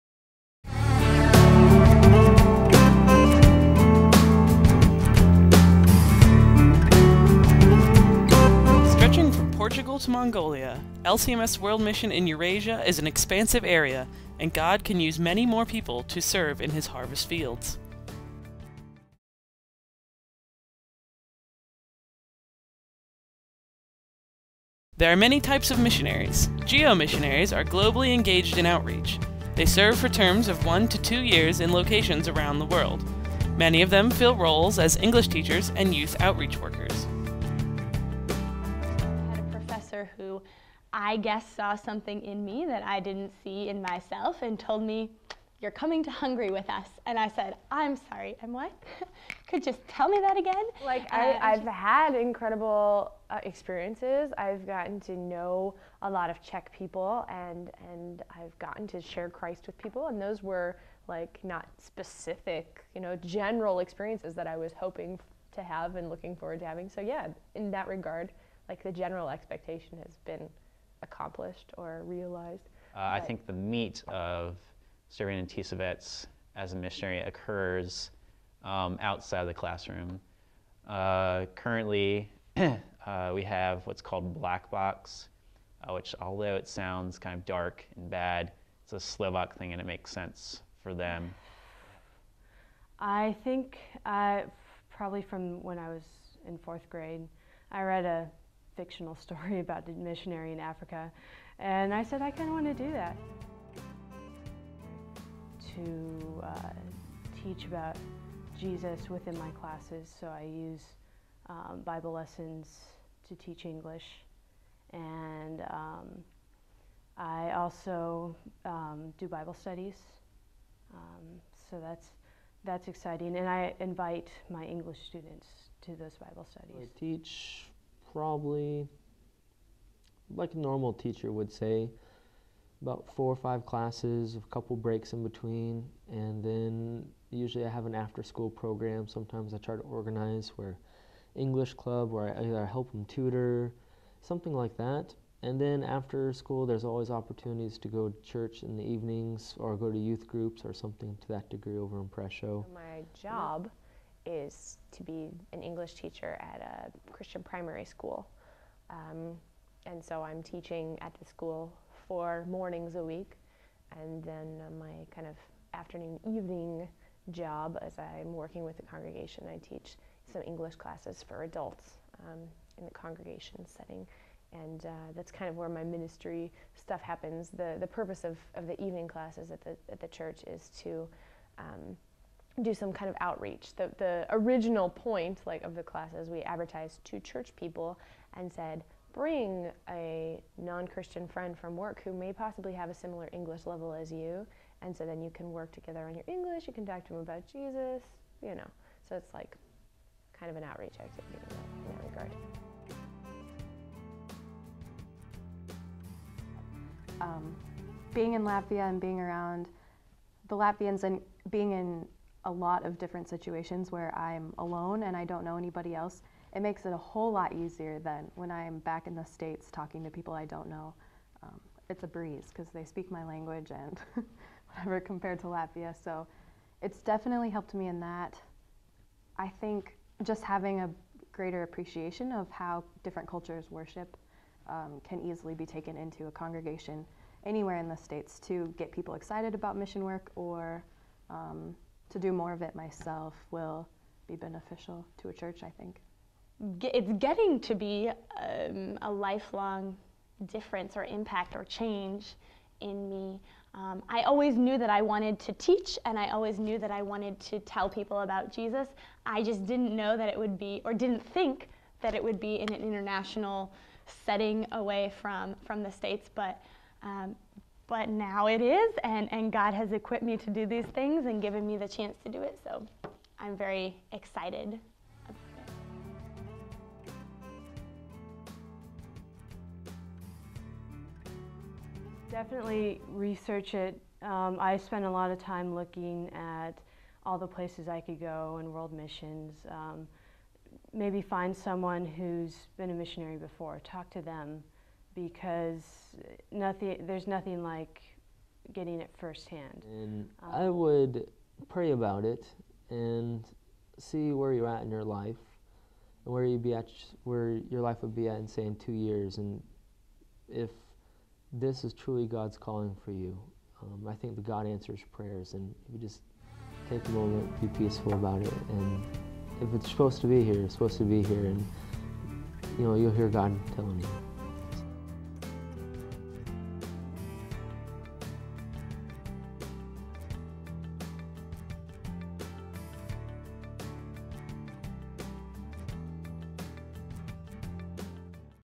Globally Engaged in Outreach Missionaries
GEO missionaries are globally engaged in outreach for 1-2 years. GEO missionaries focus on relationship ministry, often through English-as-a-foreign-language classes, but also through HIV education, homeschooling, and coordinating mission teams.